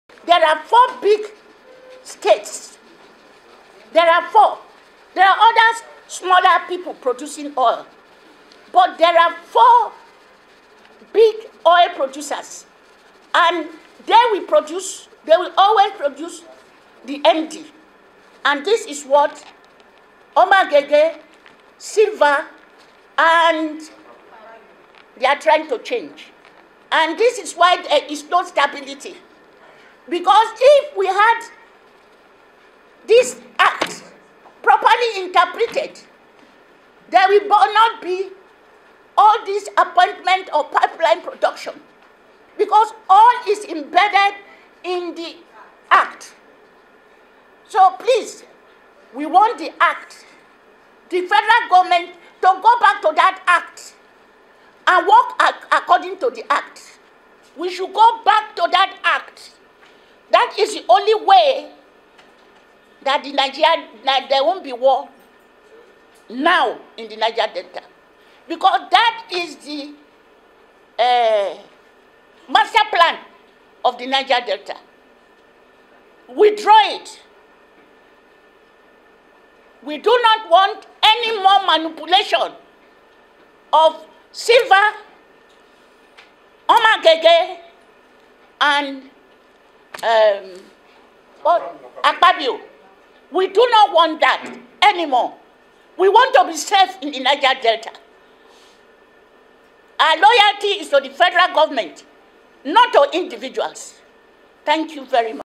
Chief Rita Lori Accuses Akpabio, Omo-Agege , Timipreye Sylva over Planting War in Niger/Delta
Review NDDC master plan act , only way Prevent War In The Region.